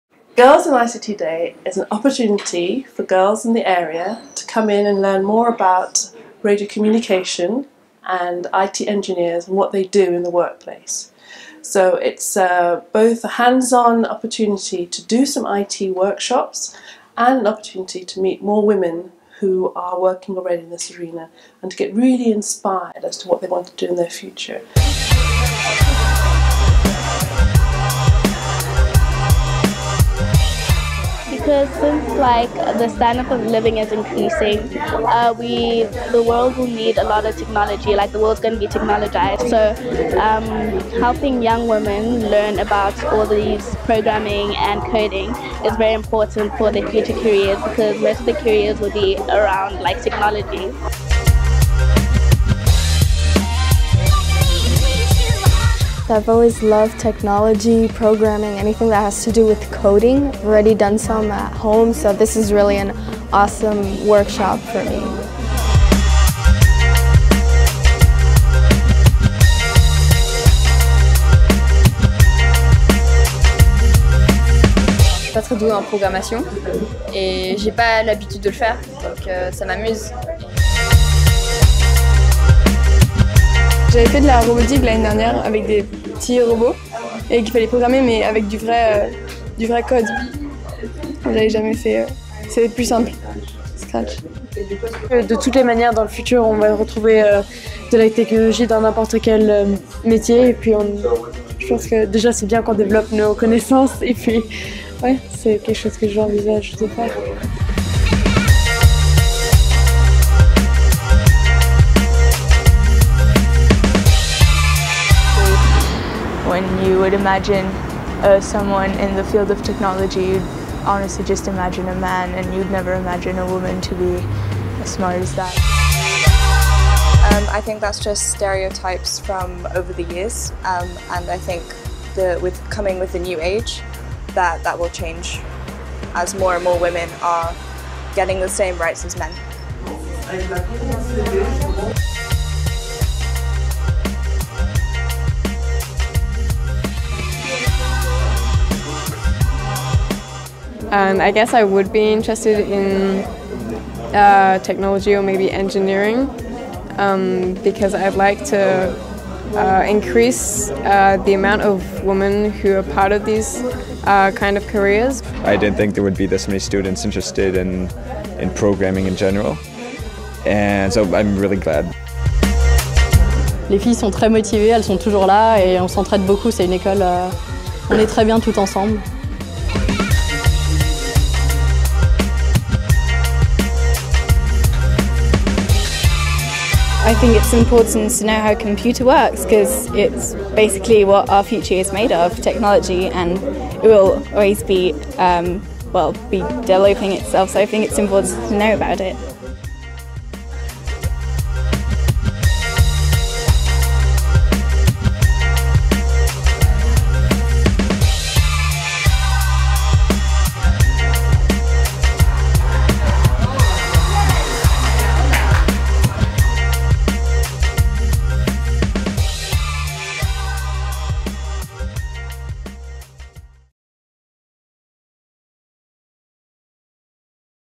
Girls In ICT Day 2016 Workshop Highlights
The Girls in ICT initiative of the International Telecommunication Union (ITU) is a global effort to raise awareness on empowering and encouraging girls and young women to consider studies and careers in Information and Communication Technologies (ICTs). The initiative is committed to celebrate and commemorate the International Girls in ICT Day on the fourth Thursday of every April as established by the ITU membership.